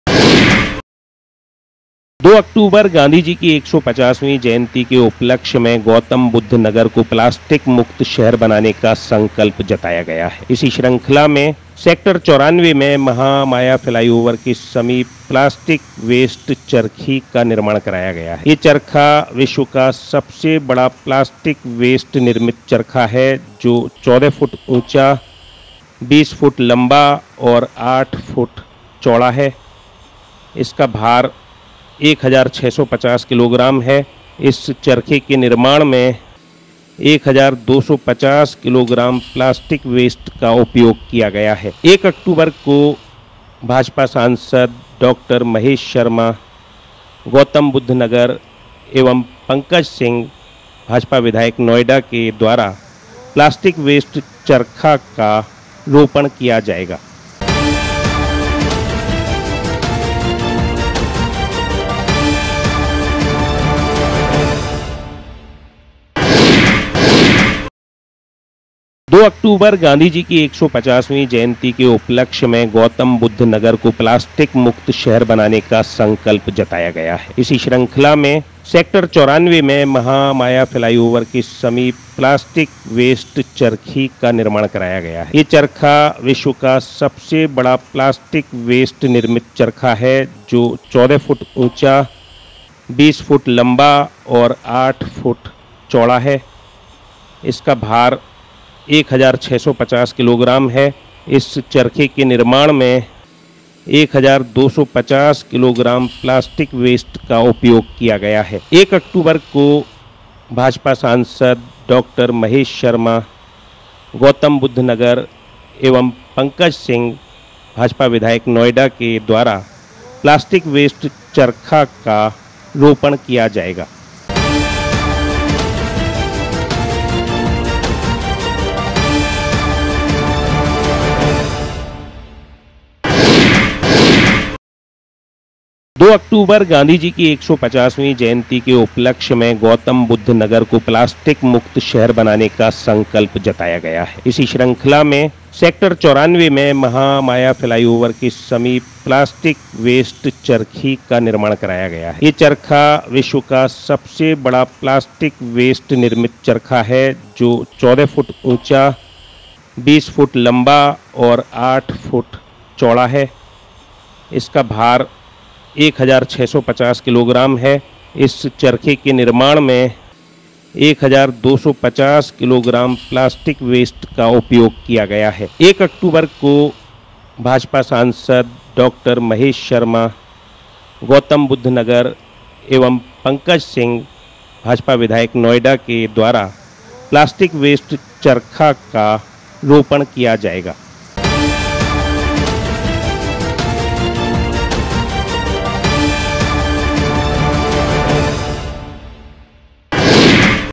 TTI NOIDA Gautam Buddh Nagar गॉंधी जयन्ती पर प्लास्टिक वेस्ट चरखे का निर्माण कर कराई स्थापना
Report Divyanjali Pandeya
यह चरखा विश्व का सबसे बड़ा प्लास्टिक वेस्ट से निर्मित चरखा है जो कि 14 फुट ऊंचा 20 फुट लंबा तथा 8 फुट चौड़ा है तथा इसका भार 1650 किलोग्राम है।
इस चरखा के निर्माण में 1250 किलोग्राम प्लास्टिक वेस्ट का उपयोग किया गया है।